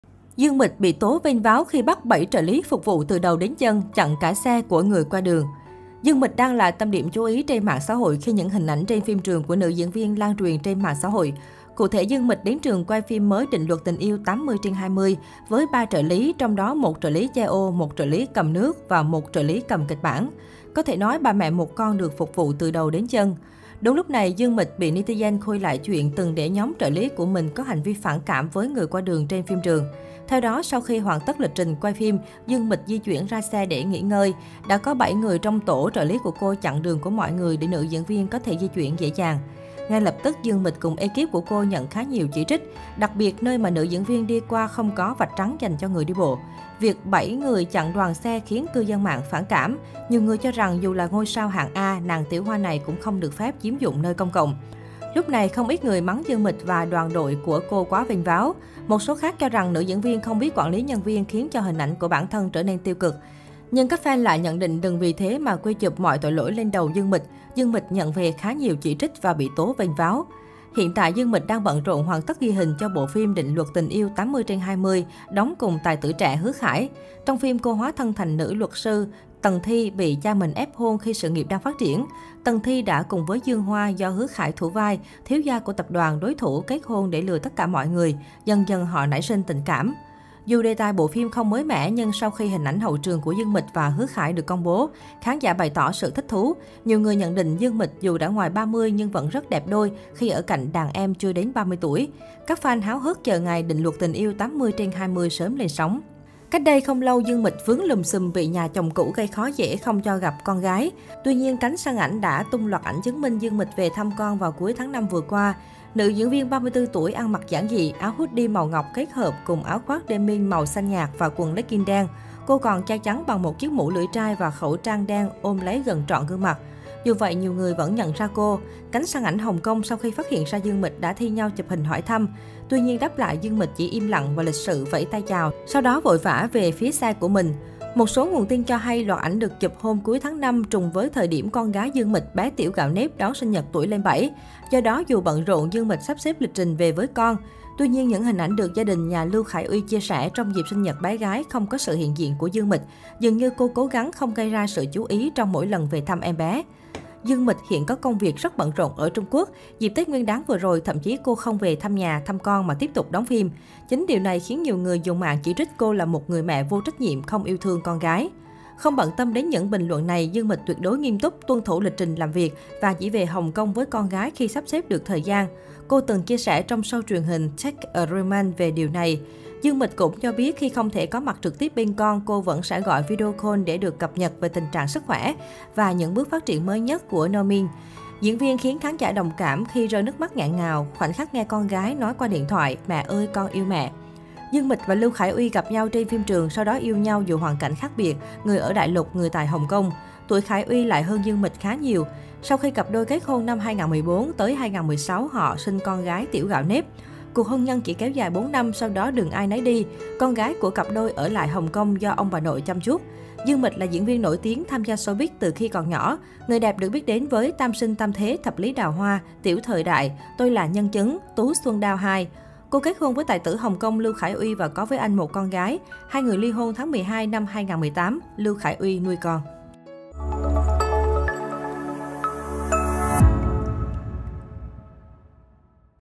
Dương Mịch bị tố vênh váo khi bắt 7 trợ lý phục vụ từ đầu đến chân, chặn cả xe của người qua đường
Từ Hy Viên đang là tâm điểm chú của cộng đồng mạng Trung Quốc sau tuyên bố ly hôn với chồng đại gia Uông Tiểu Phi sau 10 năm chung sống. Cụ thể, mỹ nhân Vườn Sao Băng xác nhận với tờ Apple Daily là vợ chồng mình đang tiến hành thủ tục ly dị. Cặp đôi không có tranh chấp tài sản, con của hai người do Từ Hy Viên hiện nuôi dưỡng tại Đài Loan.

Thông tin này khiến người hâm mộ hết sức bất ngờ vì mới ngày 22/3 vừa rồi, đại gia họ Uông đăng ảnh kỷ niệm 10 năm ngày cưới, còn tặng nhẫn kim cương cho vợ, kèm theo lời thú nhận: "Vợ, cô gái nhỏ, mừng 10 năm hoan hỷ. Anh hy vọng 10 năm sau nữa em vẫn nói với anh rằng Em đồng ý". Trong bức ảnh chụp chung, Từ Hy Viên khoe nhan sắc trẻ trung, rạng rỡ. Người hâm mộ không khỏi hoang mang, thắc mắc về nguyên nhân đường ai nấy đi của cặp đôi.